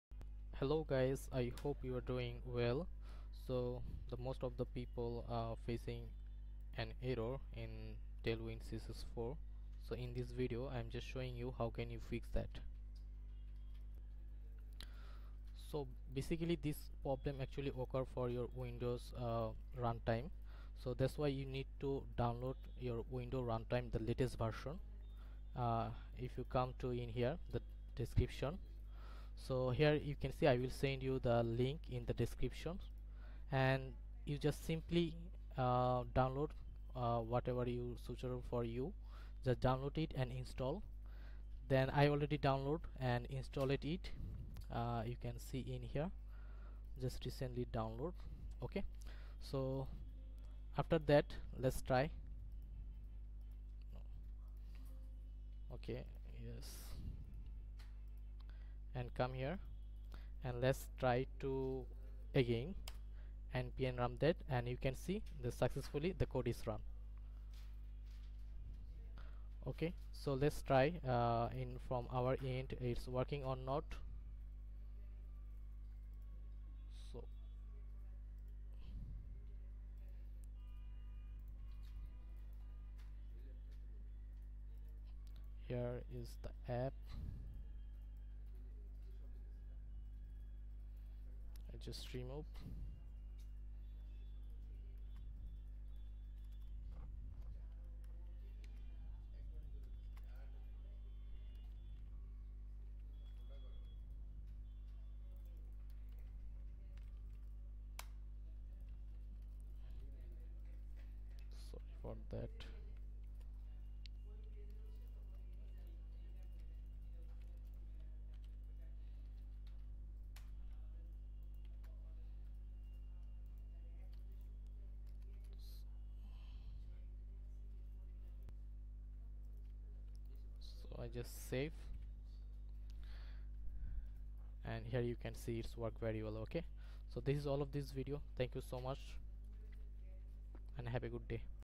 Fix Tailwind CSS version 4 error ( The specified module could not be found in vite.config.js file )
In this video we are going to learn, how to fix
Tailwind CSS version 4 The specificed module could not be found error issue.

You might be facing errors using Tailwind CSS version 4 in your React vite project.

The Error something like -
The specificed module could not be found

Hopefully the error will be fixed.